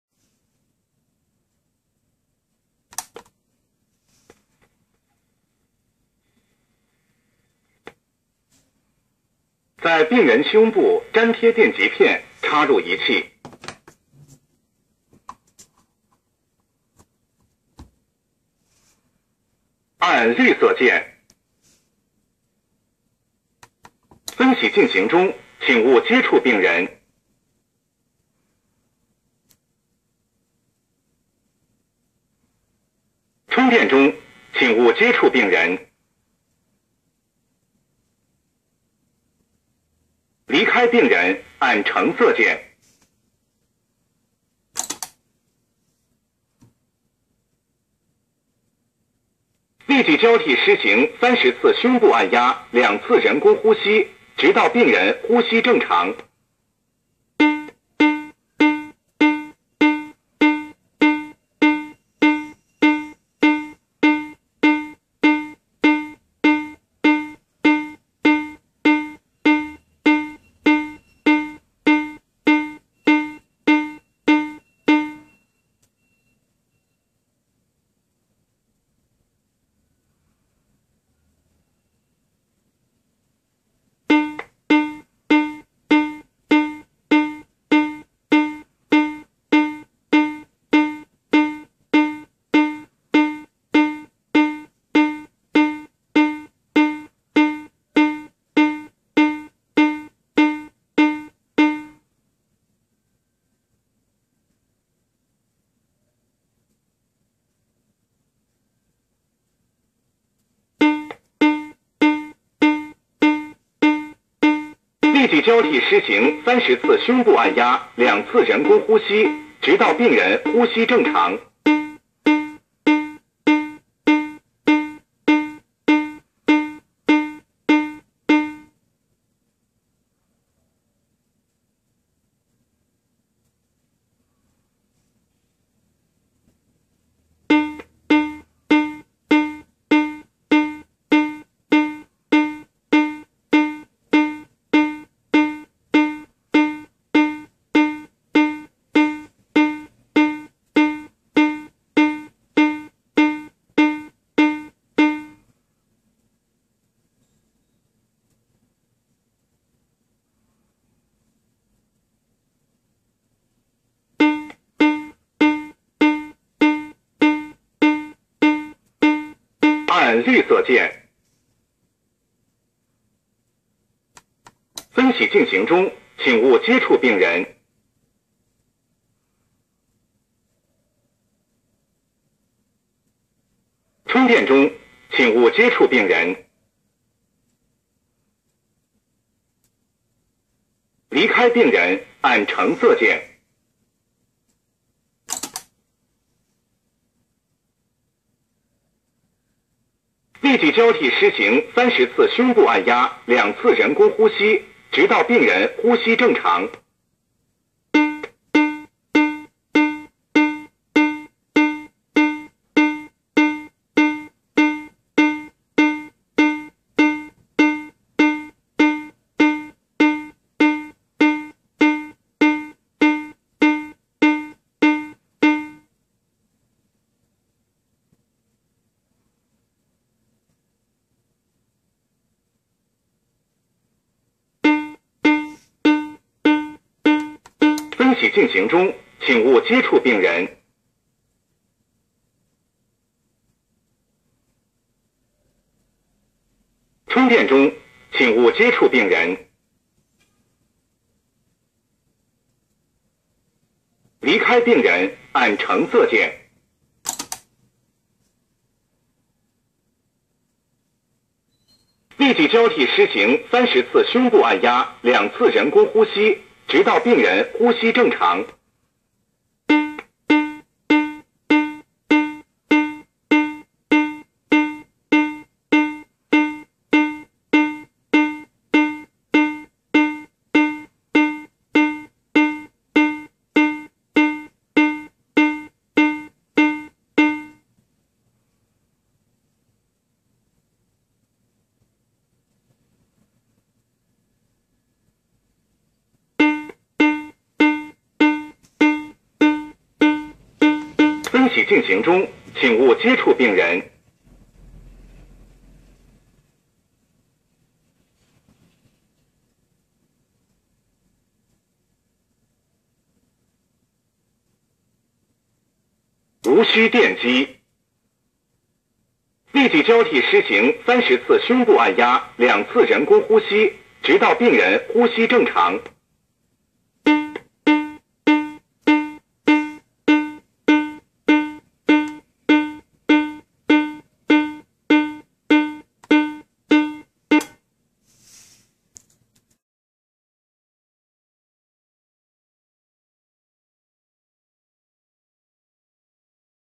Fred Easy 心臟電擊器 ( AED操作模擬 ) --德興儀器
本公司所代理AED(傻瓜型電擊器)產品
雙提示，聽覺提示與視覺提示，
聽覺提示:中文語音聲音提示，
視覺提示:可藉由LED顯示之中文字幕介面提示操作儀器